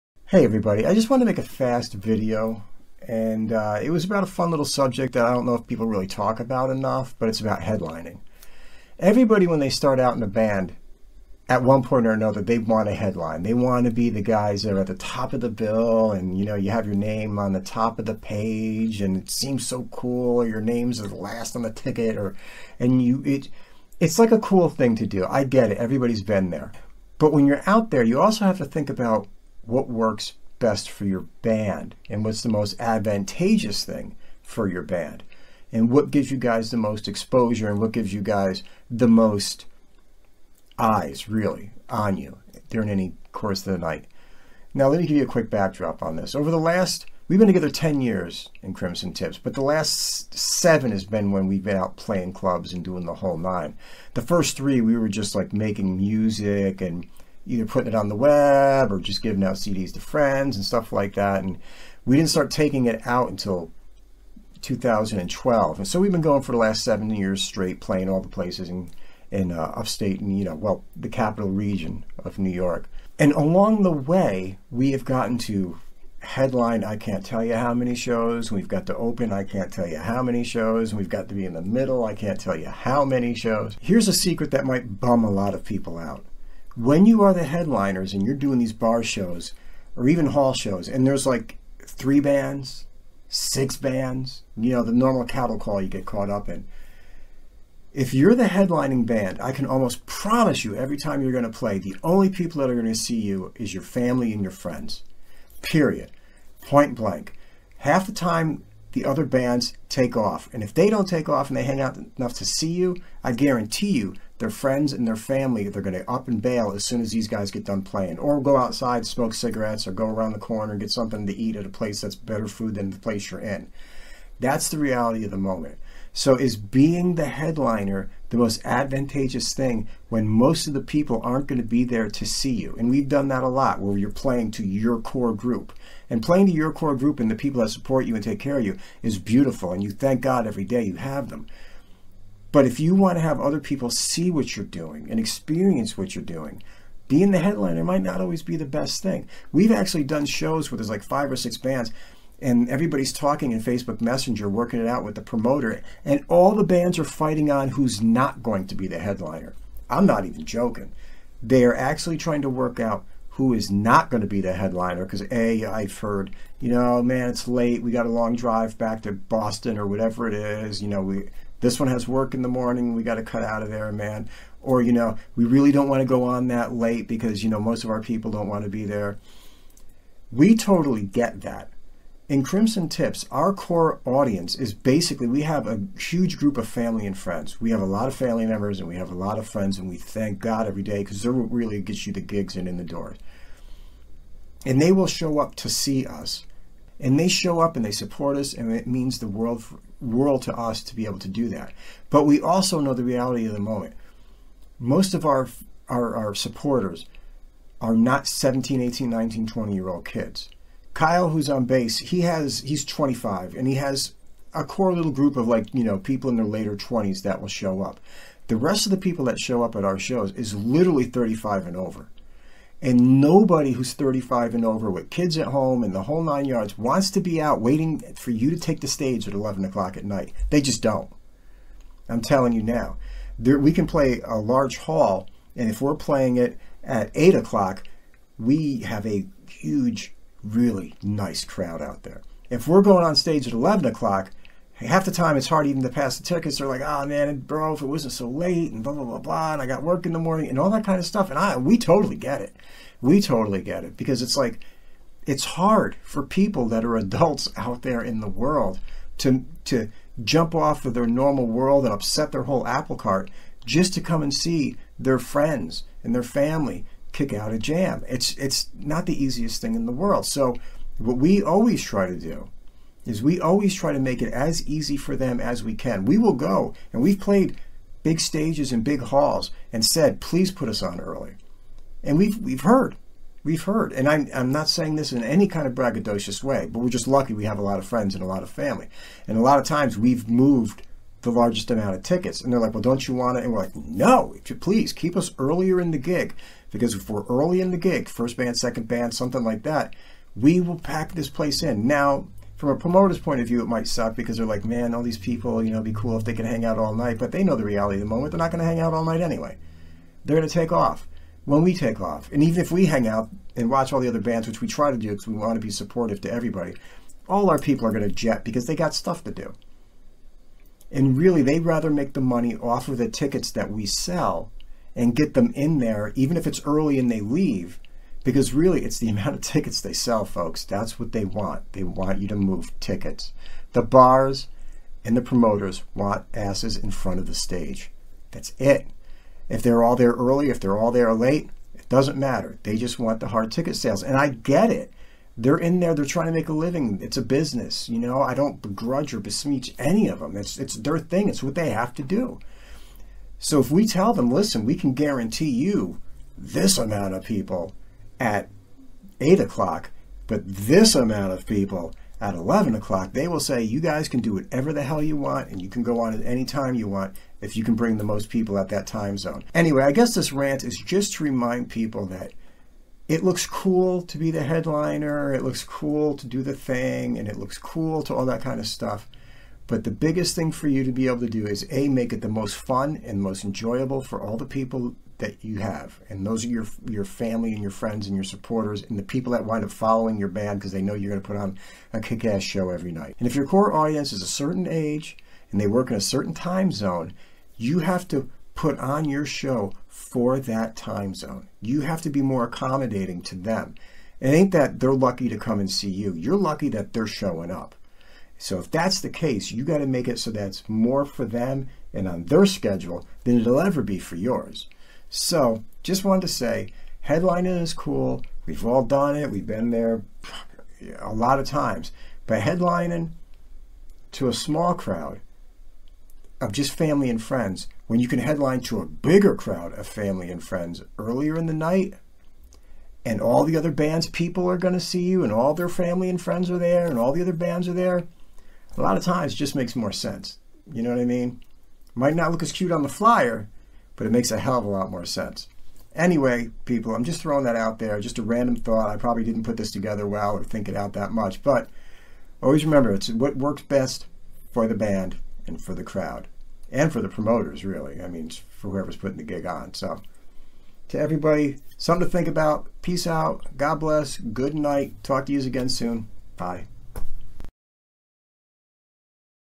HEADLINING......is it all you think it is???
Hi peoples, just a few thoughts on what it means for original bands to headline in the local music scene.